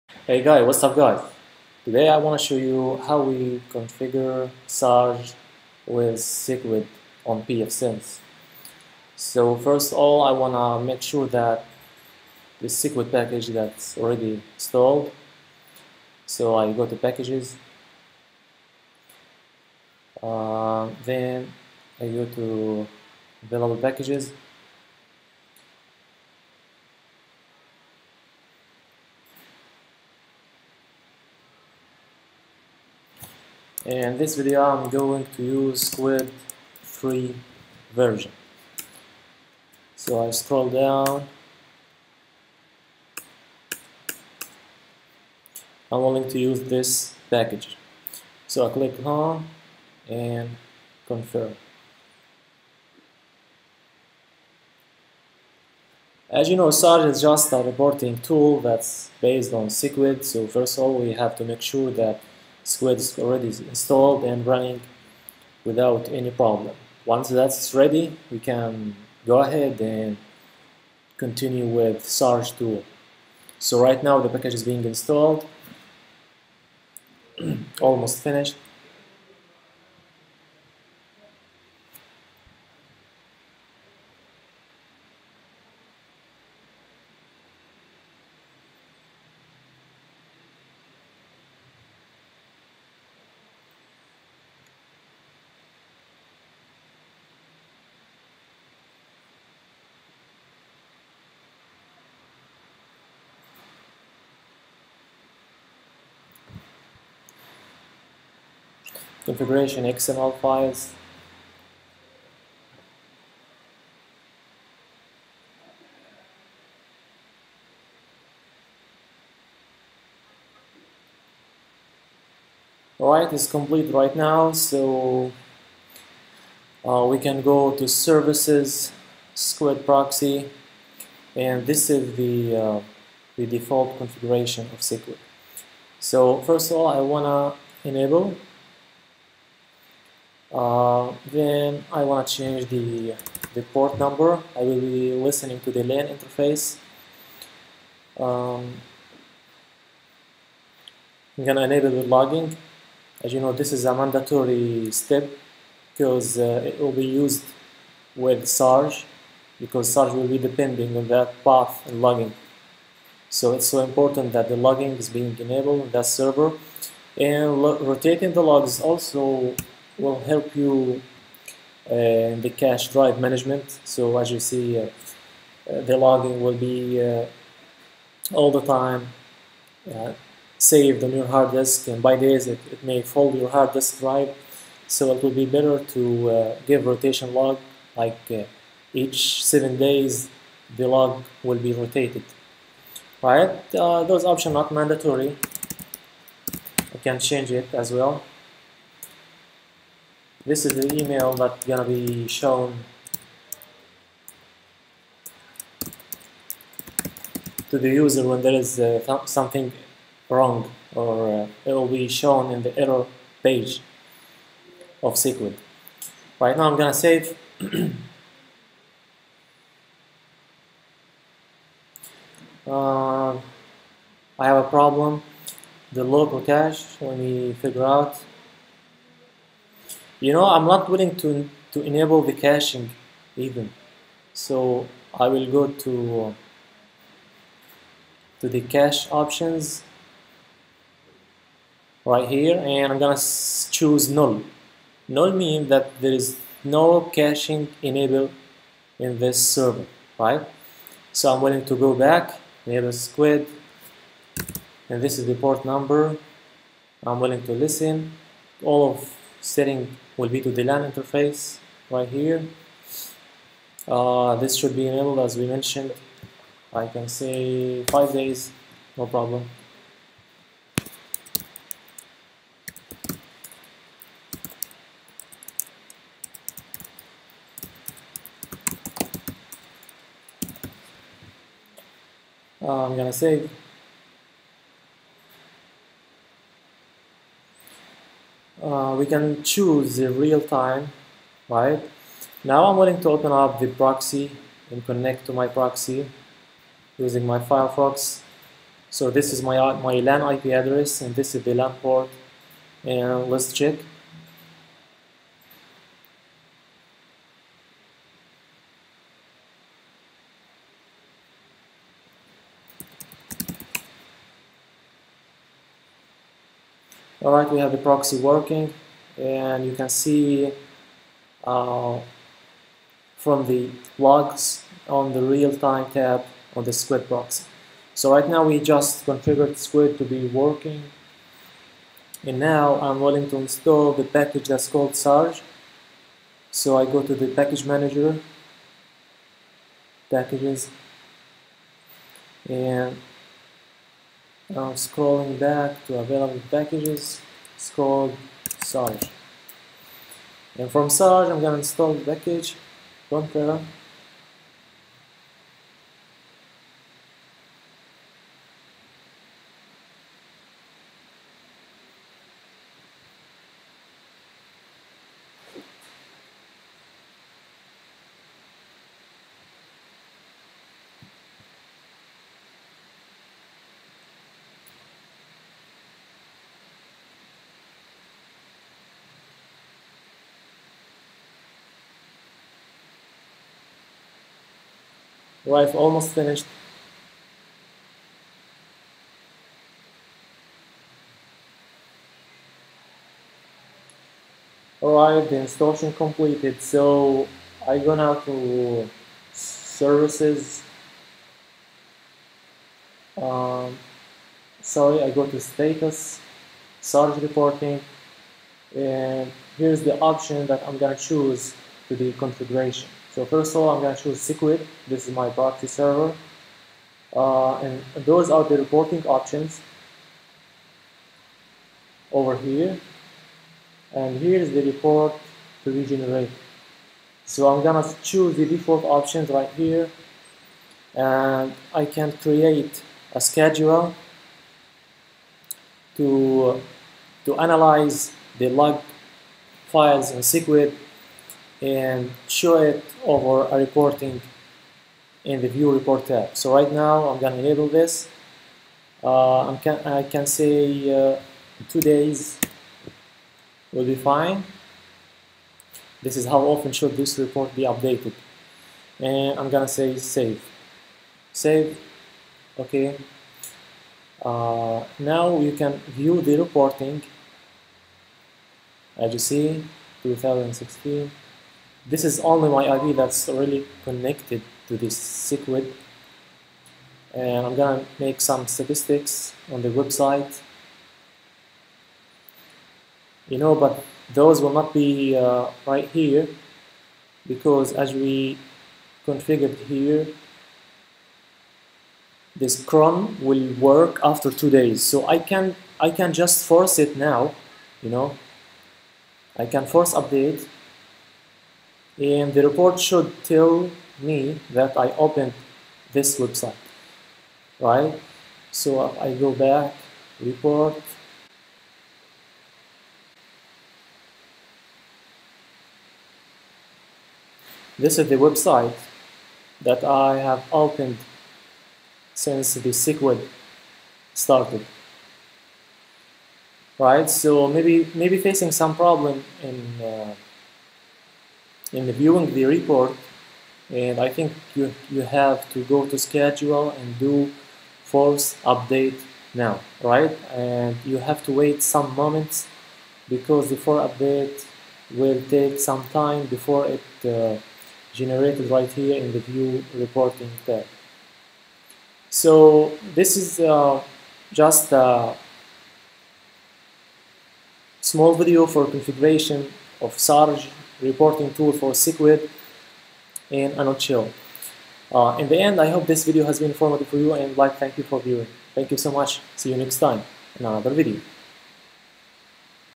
sarg reporting with squid on Pfsense firewall
i installed squid & sarg and configured sarg to read squid logs to generate the reports